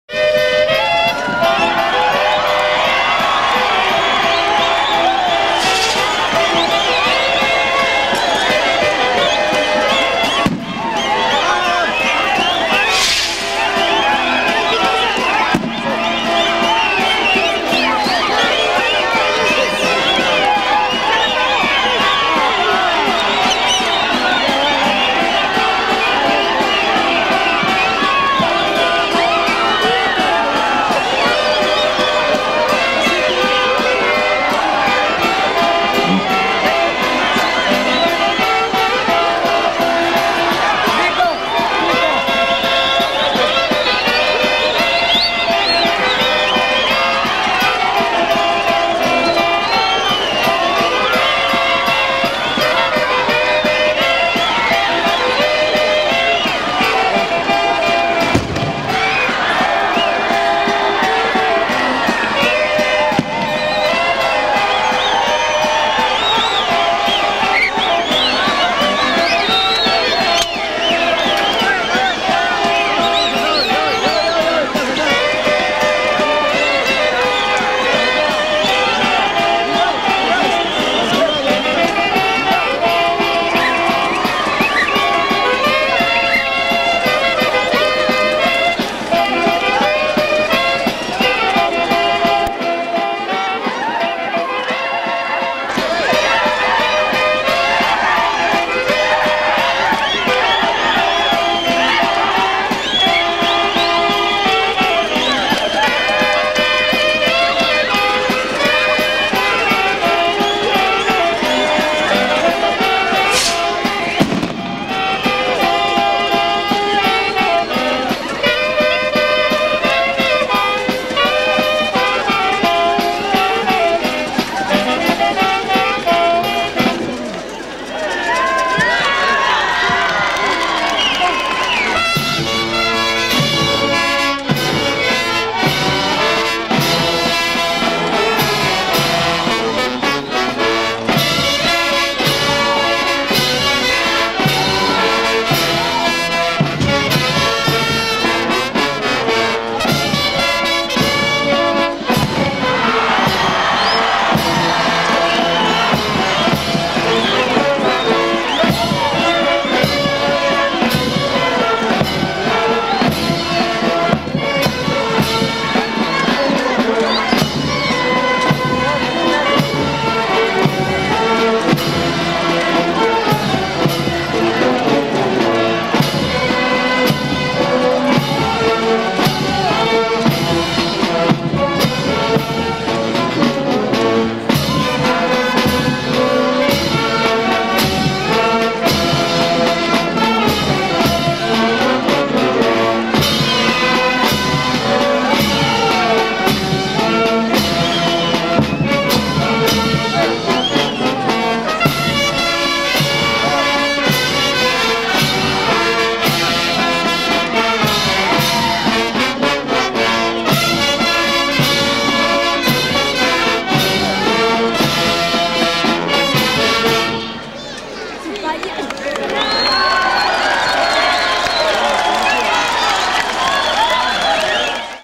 Sunicancha+Campeon 2004|Concurso de Bandas|San Pedro de Cajas|Peru
"La Nueva Show Sinfonica Sunicancha Campeón" primer puesto en el concurso de bandas en el año 2004 Mayordomo Sr. Rogelio Leon Palomino con la imagen del Apóstol San Pablo.